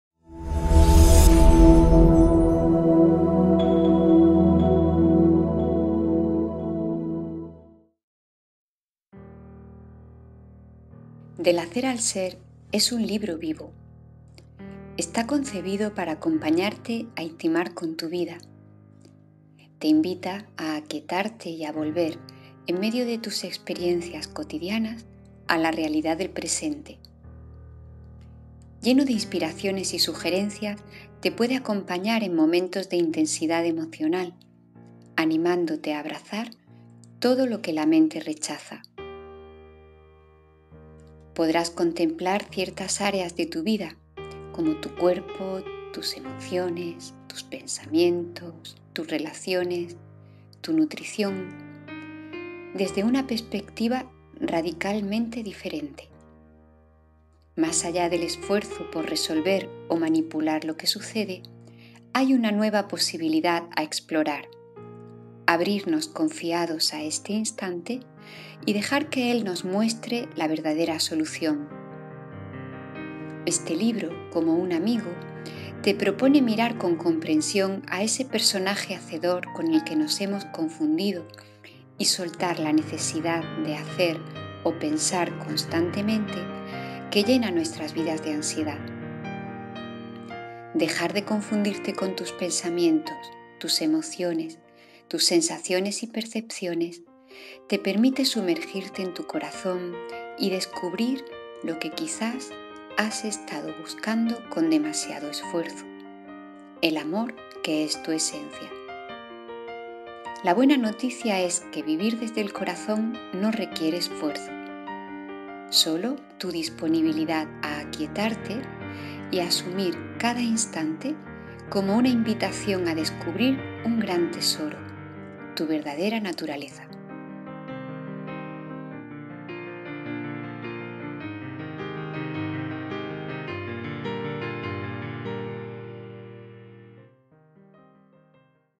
Booktrailer "Del hacer al Ser" de Dora Gil. Prólogo de Sergi Torres.
¿Te atreves a soltar el esfuerzo, a dejar de buscar la felicidad donde no se encuentra y a descansar en la paz de este instante? Si crees que es posible confiar en la vida, en lugar de temerla y evitarla; si quieres aprender a intimar con tu existencia, abrazando todo en ti... este libro te puede acompañar. Te invita a abrirte a cada experiencia cotidiana y a explorar la libertad de SER. Descubrirás el poder que vive en tu corazón.